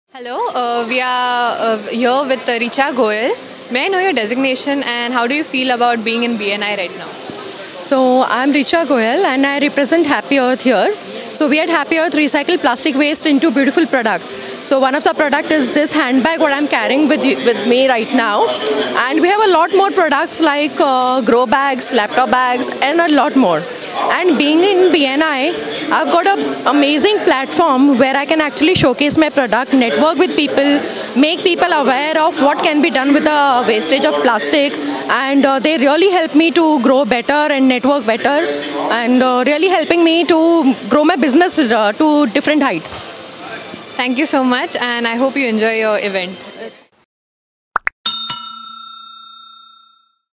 Richa Goyal Represents Happy Earth interacted exclusively with PBNBHARAT
Richa Goyal Represents Happy Earth interacted exclusively with PBNBHARAT - Goa's First Digital Business Channel. PBN BHARAT - Connect and Grow